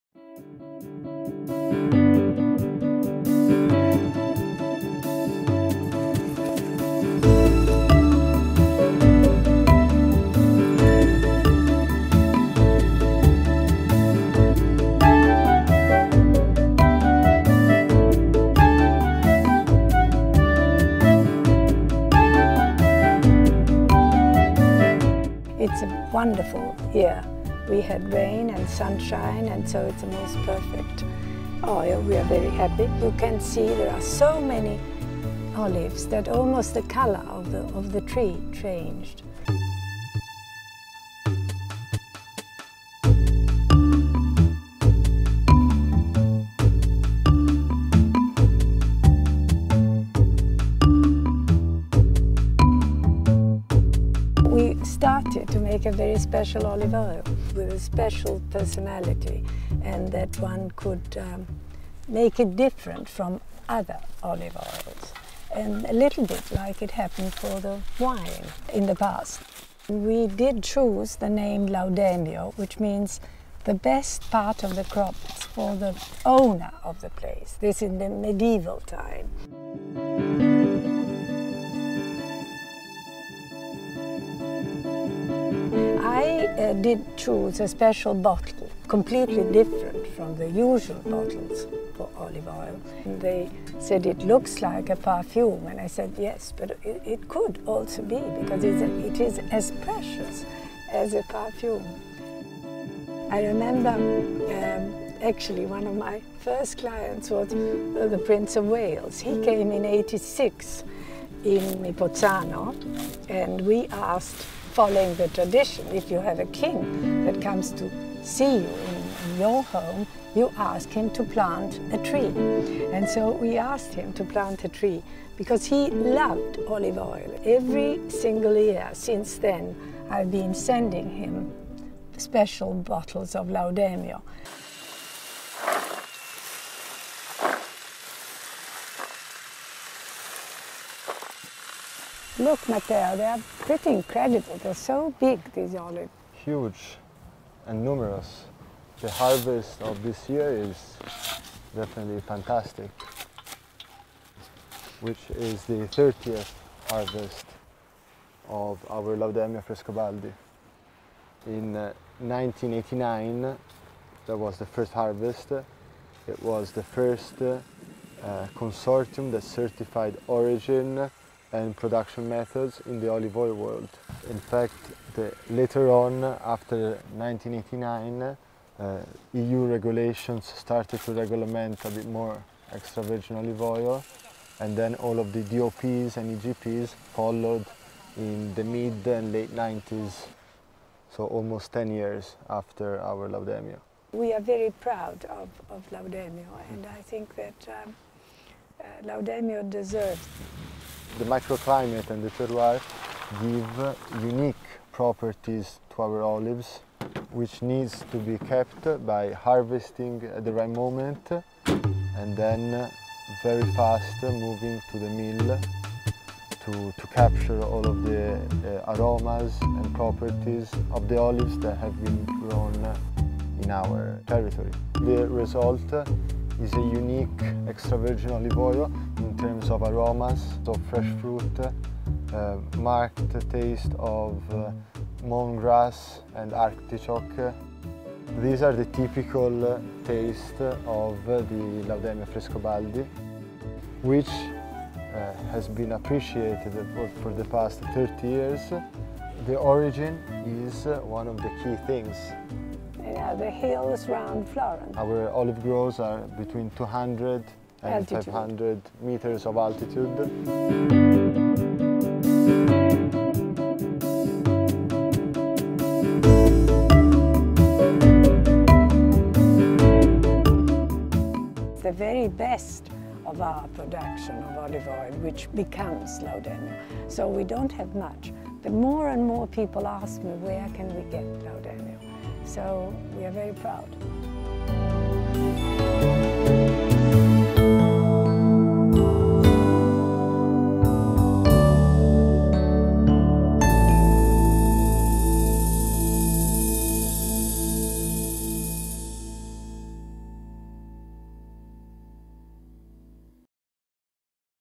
30 Emerald Green Harvests | Frescobaldi Laudemio Olive Oil | ENGLISH Version
Frescobaldi Laudemio Extra Virgin Olive Oil Celebrates its Thirtieth Harvest

The Frescobaldi family’s vision is a guarantee of Tuscan excellence, specifically their highly-regarded extra virgin olive oil. An idea that started thirty years ago has now dominated the olive oil market across the world. To celebrate its thirty years of success, Laudemio Frescobaldi has created a celebratory packaging where the traditional octagonal bottle has been painted gold, highlighting the brilliance of its fine, elegant lines.